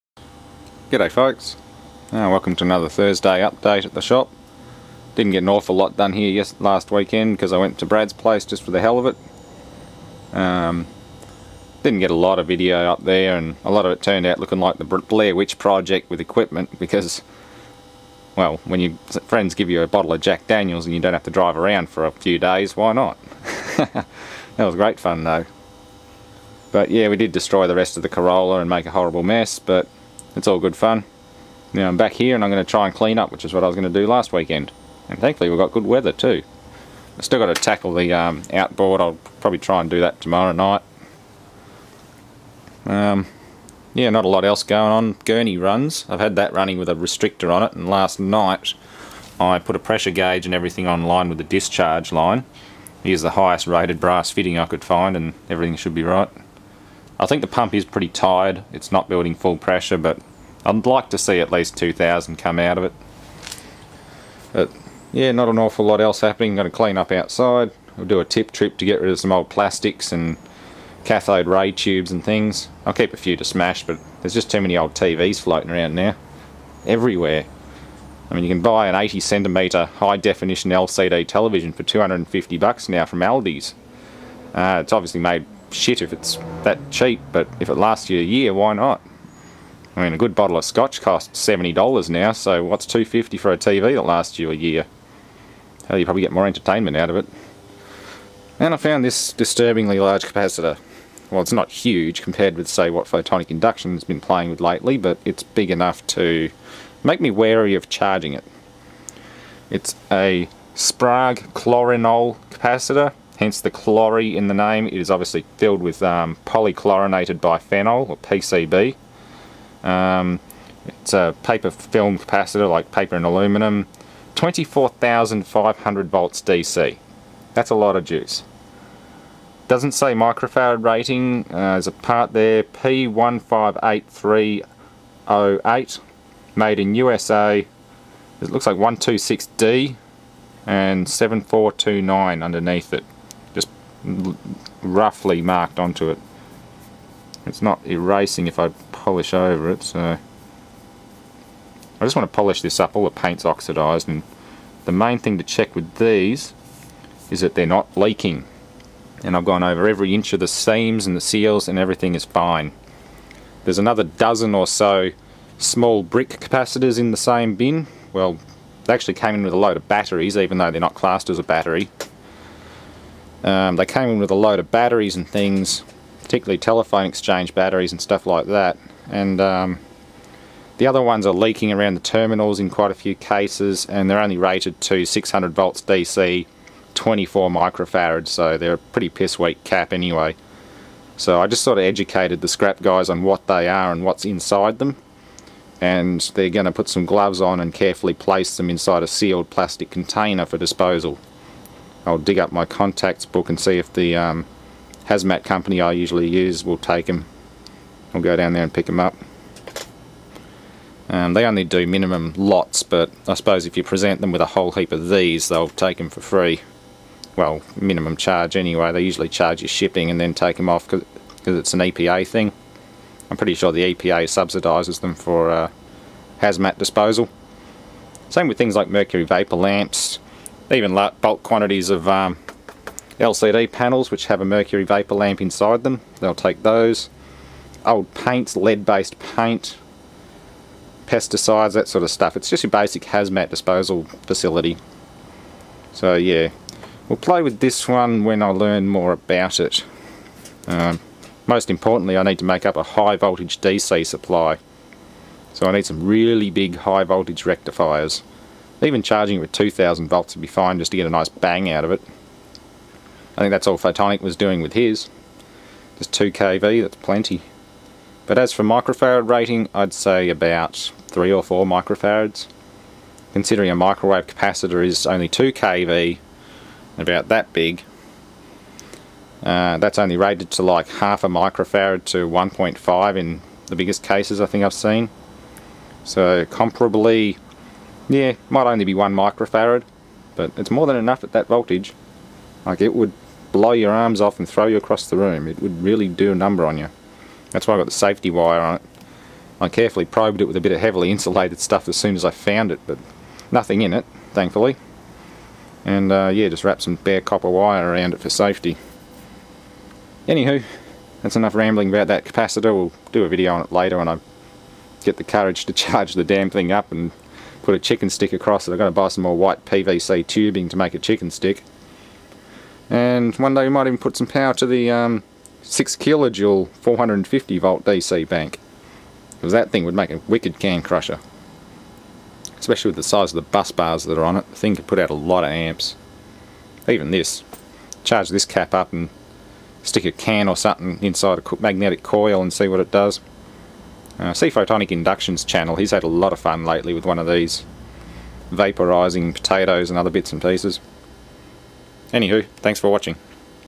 Thursday Update and Big 24000V Capacator
Real problem is there is no way to determine the polarity of the cap, no markings on the case :(
without that it may as well be a door stop